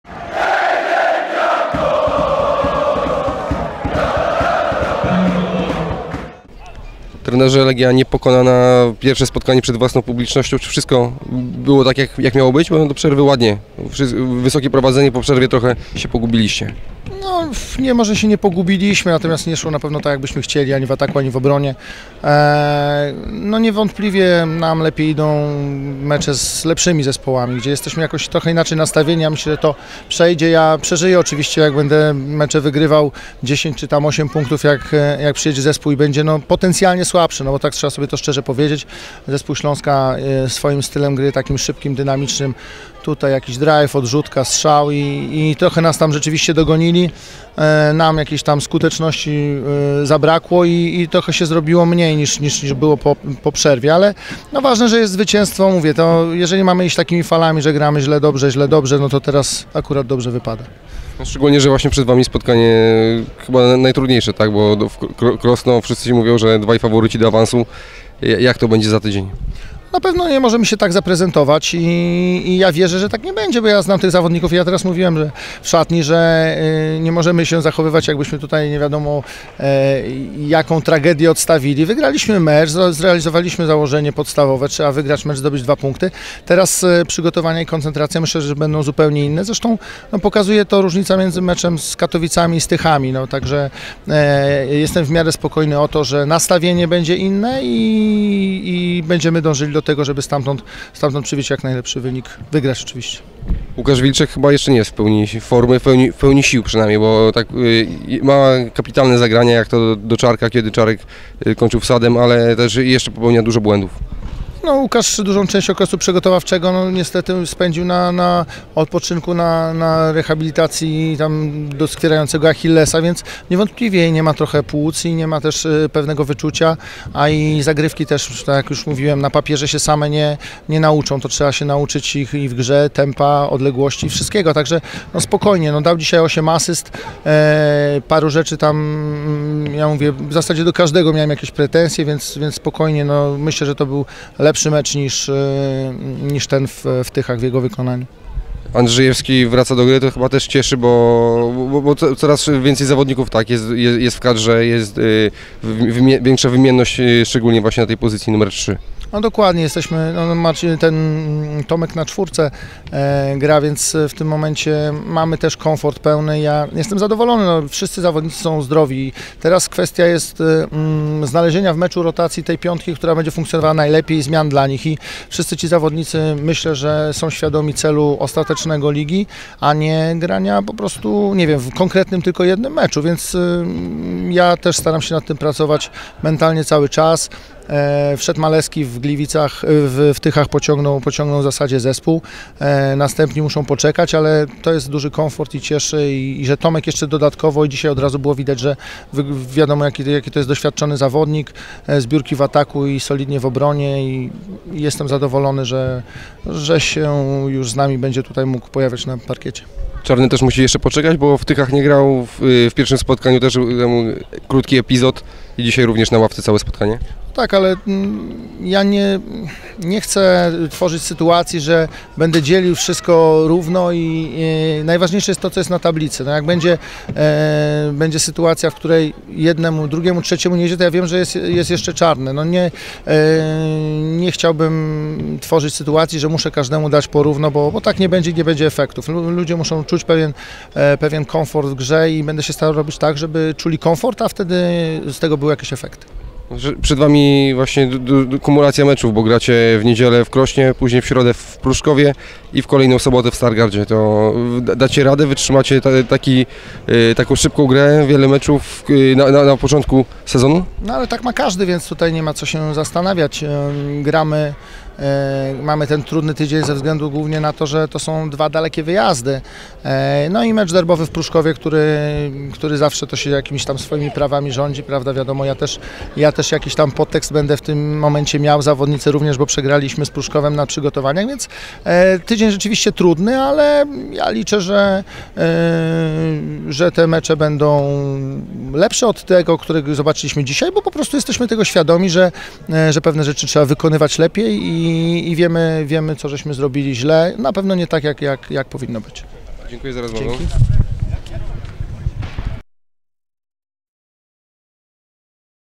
Pod koszem: Michał Spychała
Rozmowa z trenerem Legii Warszawa, Michałem Spychałą, po wygranym meczu ligowym ze Śląskiem II Wrocław (69:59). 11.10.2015